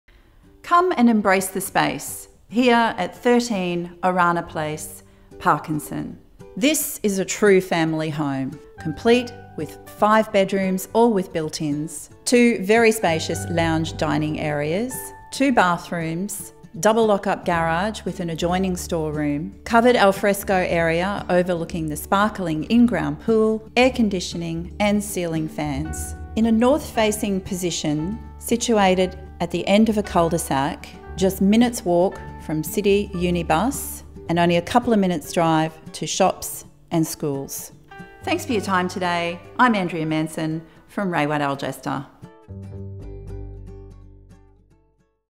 13 Orana Place, Parkinson Queensland By Andrea Manson
Property Video shoot of 13 Orana Place, Parkinson Queensland by PlatinumHD for Ray White Algester